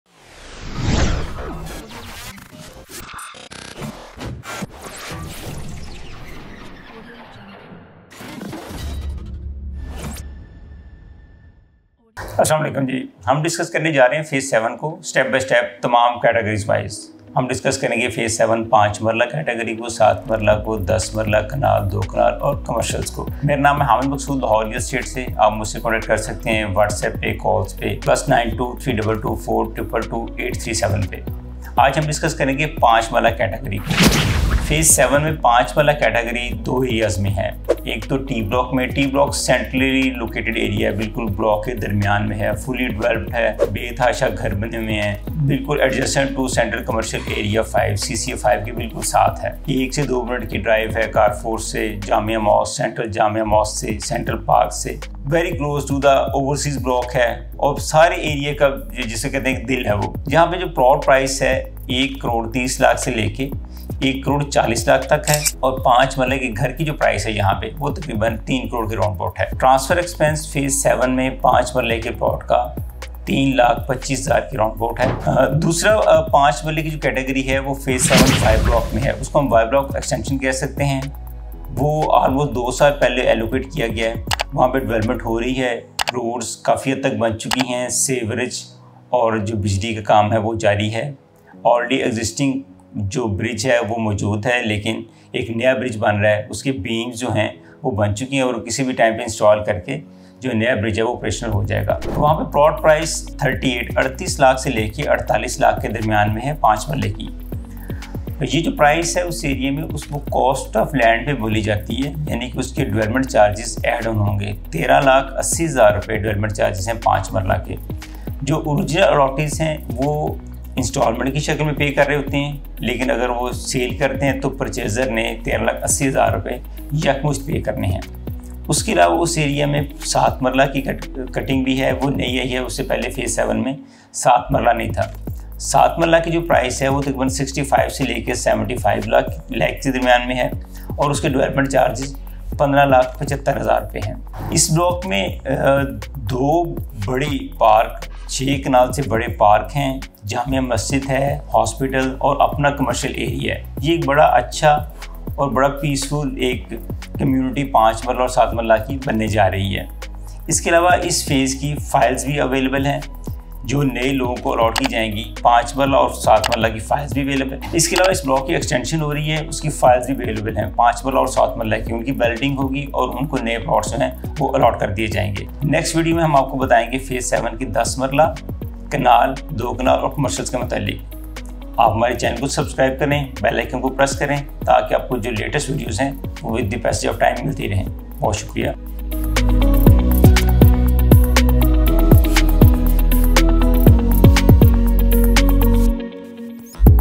DHA Lahore Phase 7 - Investing in 5 Marla Plots (Price, Development & Benefits)
This video dives deep into everything you need to know about investing in 5 Marla plots within DHA Lahore Phase 7. We'll break down the current price range, giving you a clear picture of affordability. We'll also explore the ongoing development progress in the area, showcasing the infrastructure and amenities that will enhance your investment. Finally, we'll uncover the numerous benefits of owning a 5 Marla plot in this prestigious location, including potential capital gains and a high-quality living environment.

LAHORE PAKISTAN